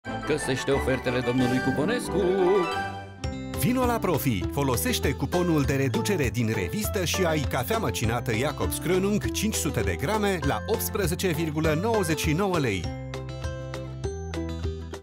Foloseste cuponul de reducere din revista si ai Cafea măcinată Jacobs Krönung la un pret special.
Vino la PROFI, foloseste cuponul de reducere din revista si ai Cafea măcinată Jacobs Krönung 500g la un pret special. Oferta supusa unor termene si conditii. Profi, zilnic preturi mici.